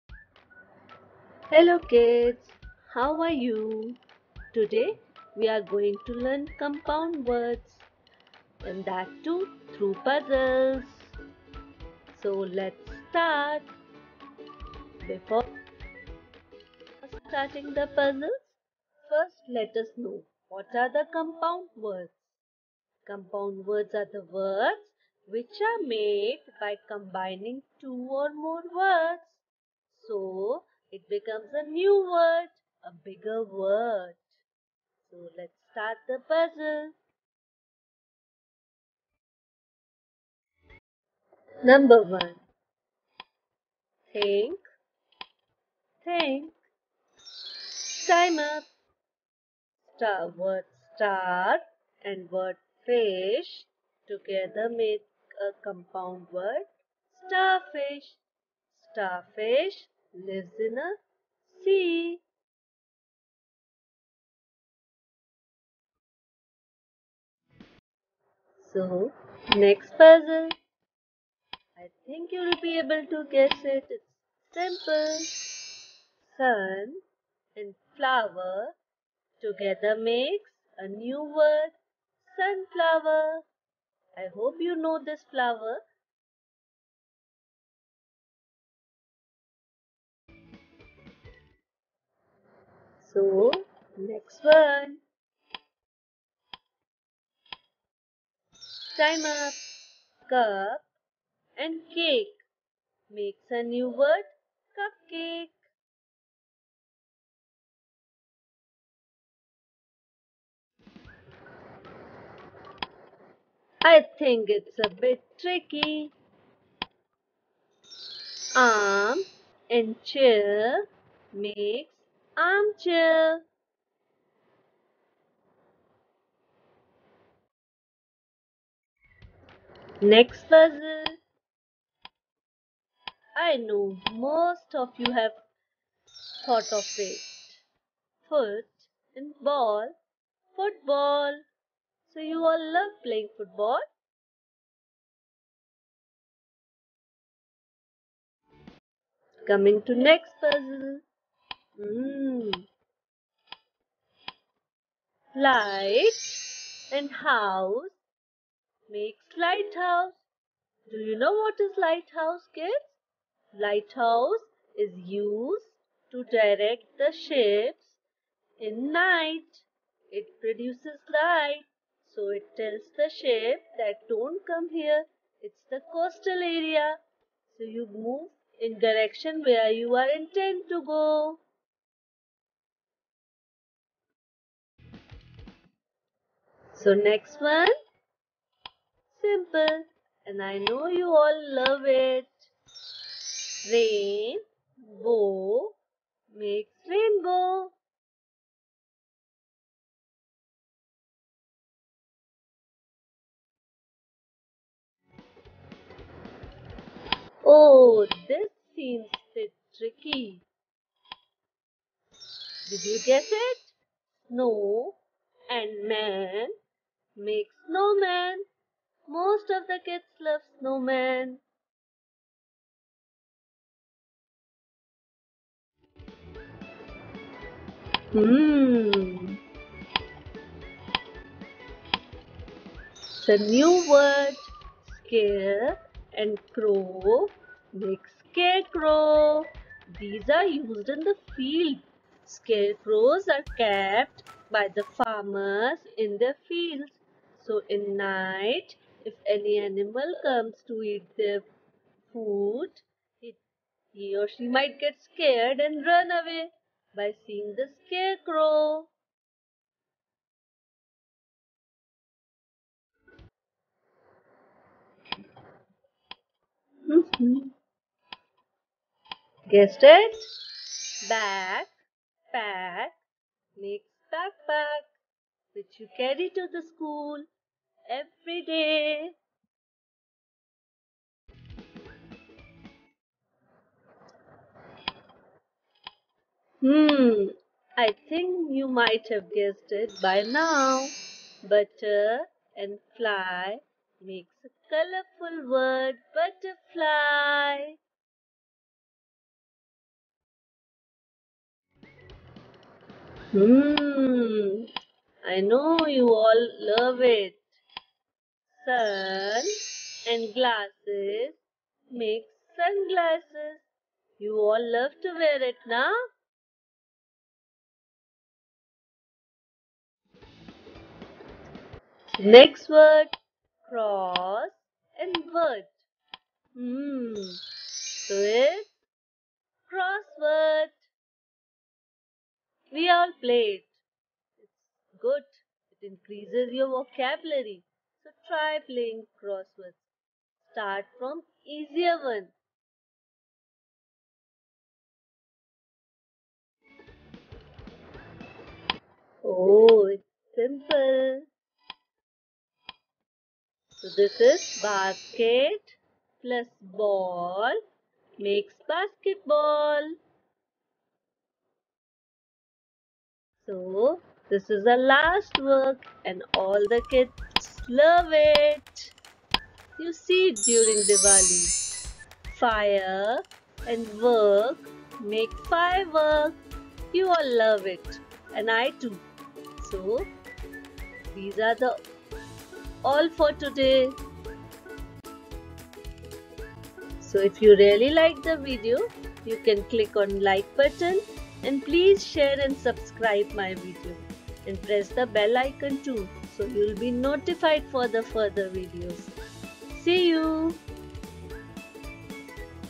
Compound Words | Examples of compound words | Bright Kidzz
Compound Words | Examples of compound words

Hello kids, Welcome to Bright Kidzz channel. In this video, I will be covering Compound Words in form of quiz.

Compound words are words which  are formed by combining 2 words.

eg in + side = inside
back + pack = backpack

Happy Learning!